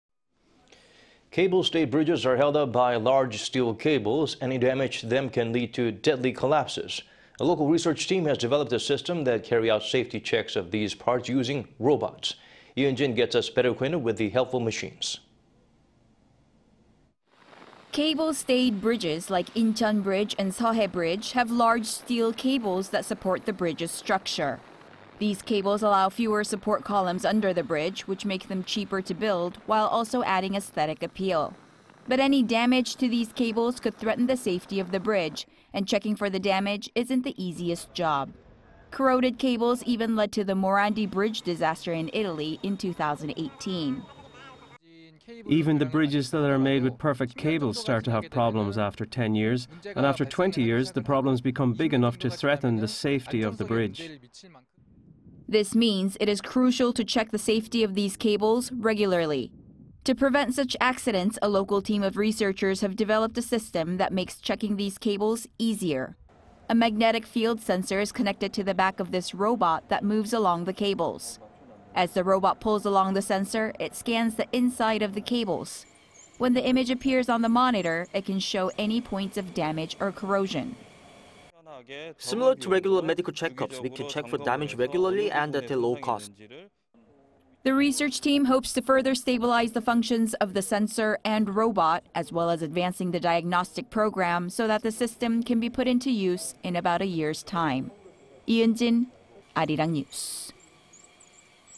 Checking safety of cable-stayed bridges with robot sensors
로봇과 센서로 사장교 케이블 안전 진단

Cable-stayed bridges are held up by large steel cables.
Any damage to them can lead to deadly collapses.
A local research team has developed a system that carry out safety checks of these parts using robots.
Lee Eun-jin gets us better acquainted with the helpful machines.

Cable-stayed bridges like Incheon Bridge and Seohae Bridge have large steel cables that support the bridge's structure.
These cables allow fewer support columns under the bridge which makes them cheaper to build, while also adding aesthetic appeal.
But any damage to these cables could threaten the safety of the bridge, and checking for the damage isn't the easiest job.
Corroded cables even led to the Morandi Bridge disaster in Italy in 2018.

"Even the bridges that are made with perfect cables start to have problems after 10 years, and after 20 years, the problems become big enough to threaten the safety of the bridge."

This means it is crucial to check the safety of these cables regularly.

To prevent such accidents, a local team of researchers have developed a system that makes checking these cables easier.
A magnetic field sensor is connected to the back of this robot that moves along the cables.
As the robot pulls along the sensor, it scans the inside of the cables.
When the image appears on the monitor, it can show any points of damage or corrosion.

"Similar to regular medical checkups, we can check for damage regularly and at a low cost."

The research team hopes to further stabilize the functions of the sensor and robot
as well as advancing the diagnostic program so that the system can be put into use in about a year's time.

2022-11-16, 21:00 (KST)